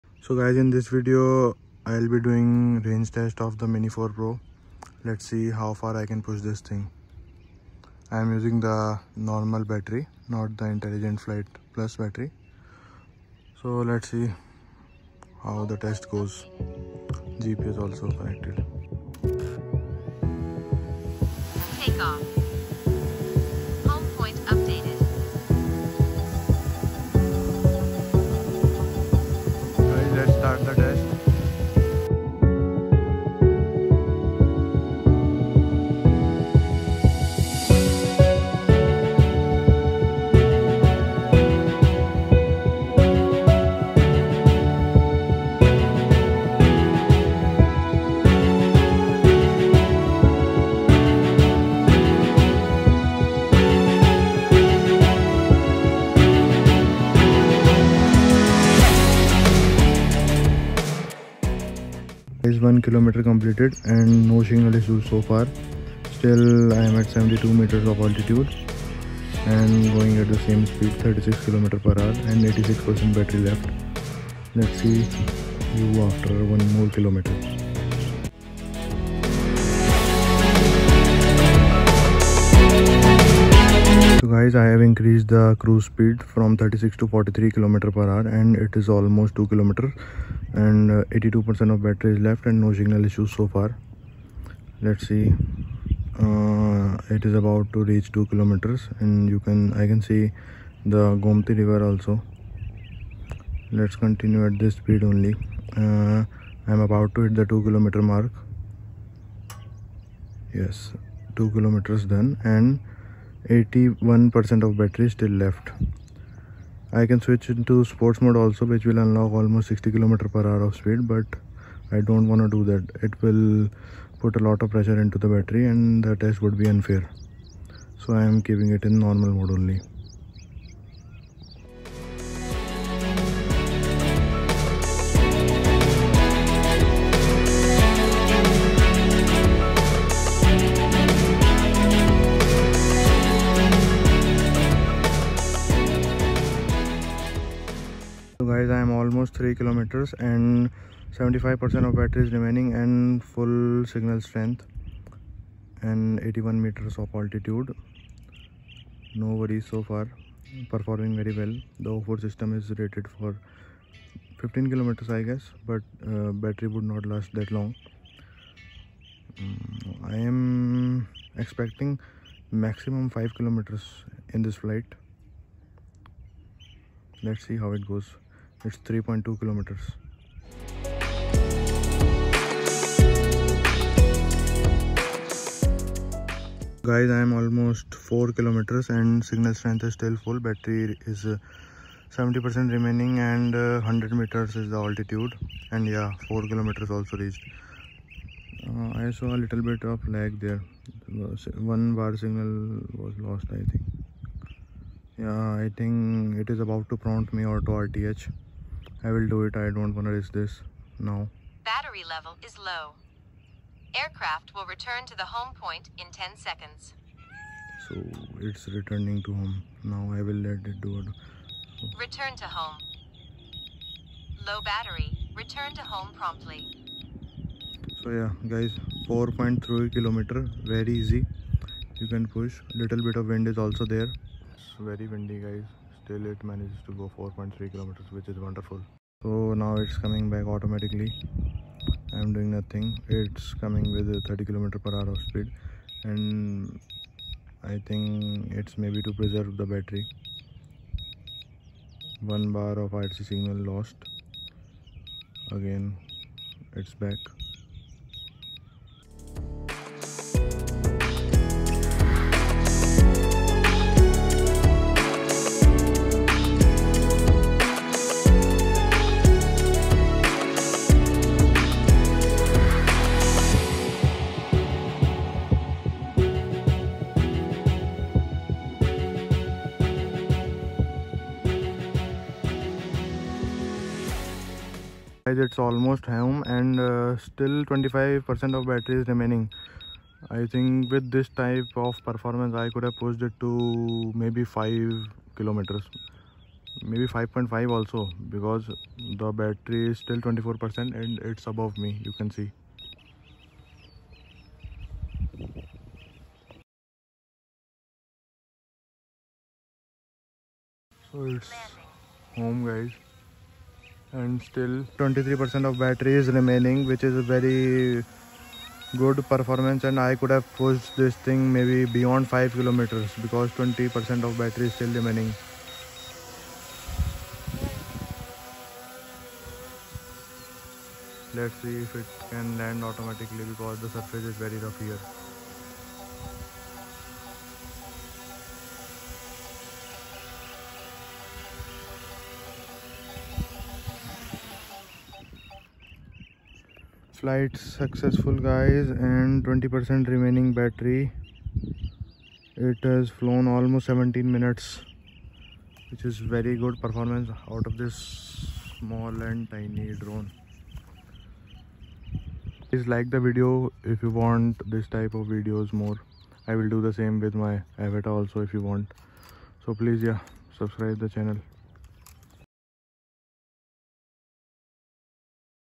DJI Mini 4 Pro Range Test | Insane Range | 4K | 15000Ft/4KM +
Pushing the DJI Mini 4 Pro to its limits to explore its maximum range capability in a real-world scenario. Shot in stunning 4K, this video demonstrates the Mini 4 Pro’s Ocusync 4 transmission performance as it flies over 15,000 feet (4.5 kilometers) while maintaining a strong signal and video feed.